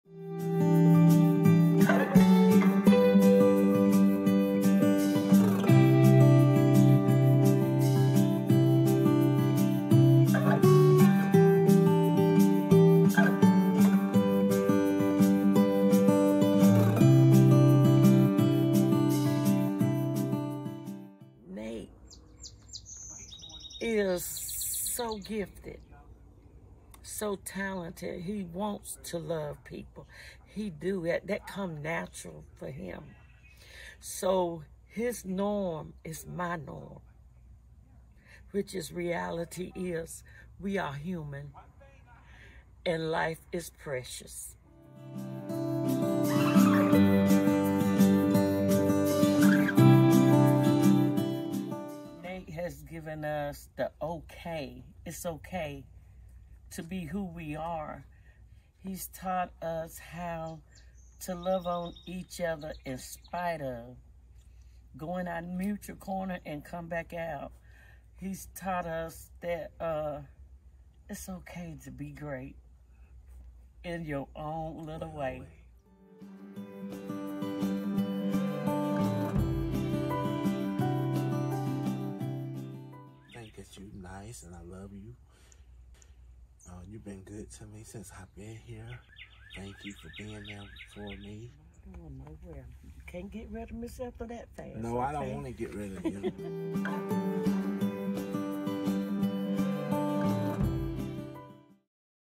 AFL Model Fosters Personalized Home Care
Winston “Nate” Simmons clearly remembers May 15, 2020, like it was yesterday. It was moving day, and he packed his belongings - clothes, shoes and books - to move from a group home to his Direct Support Professional Ethel Bridges' house. Ethel would now serve as his primary caregiver through Monarch’s Alternative Family Living (AFL) service. Learn more about AFL and how Nate feels about his personalized, compassionate care. Also visit here, for more details about Nate's story